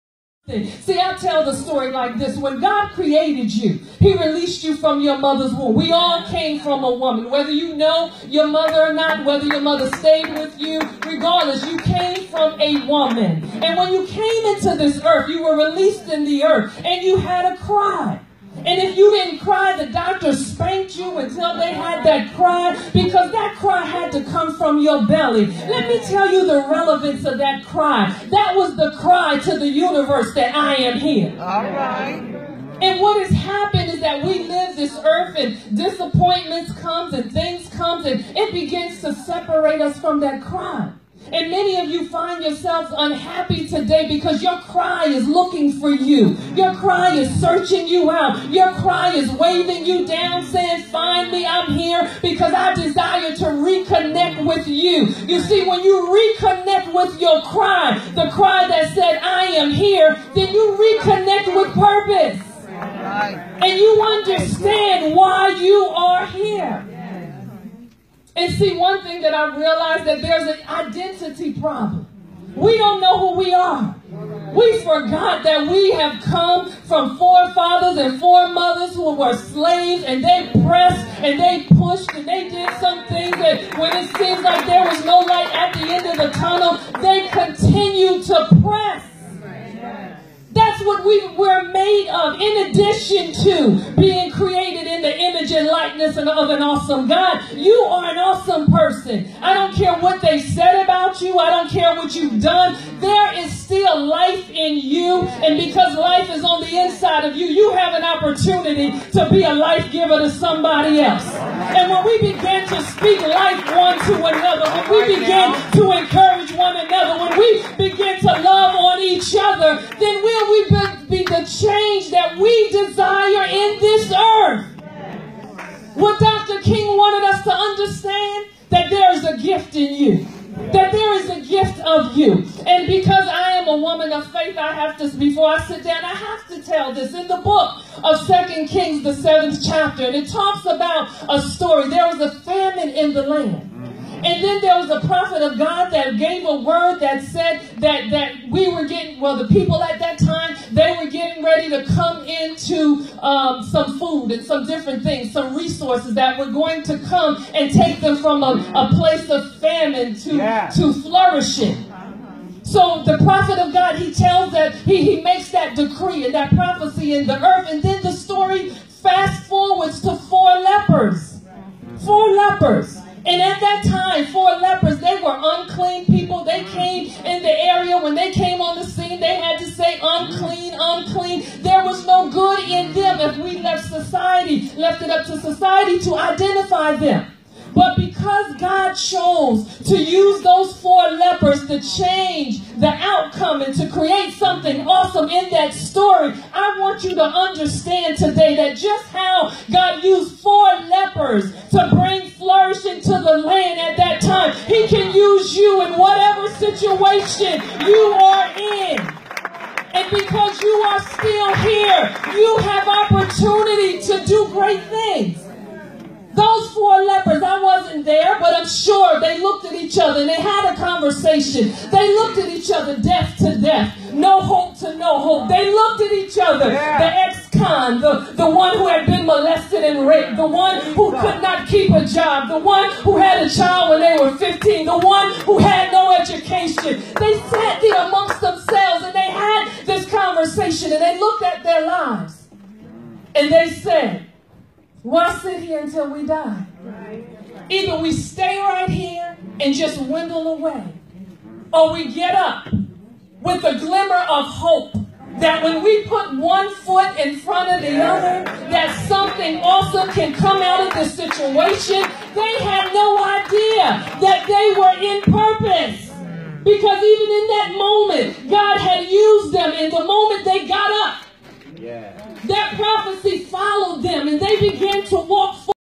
MLK Jr Program
January 16, 2017 Martin Luther King, Jr. birthday celebration at the Masonic Temple, Progressive Lodge #17 - New Brunswick